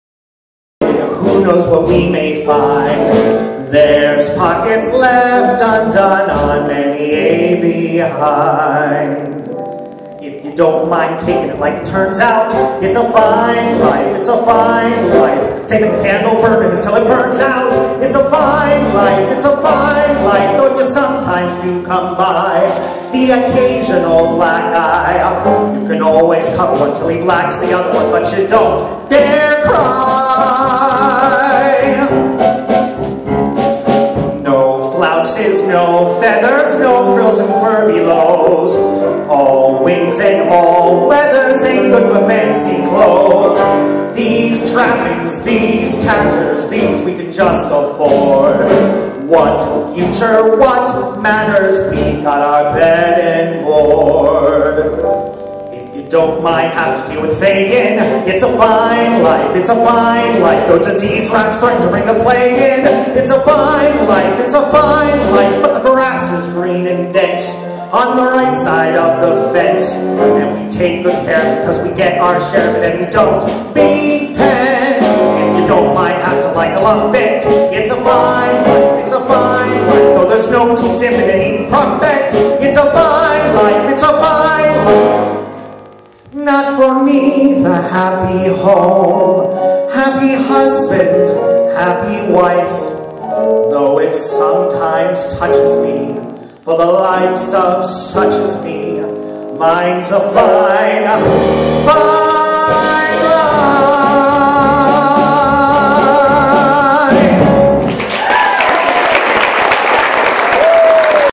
Eric Millegan
sorry it's not an actual video, i had to keep my phone in my pocket so my dad wouldn't get mad. omg just listen to him, how perfect he sounds. this show was absolutely amazing. eric is so talented!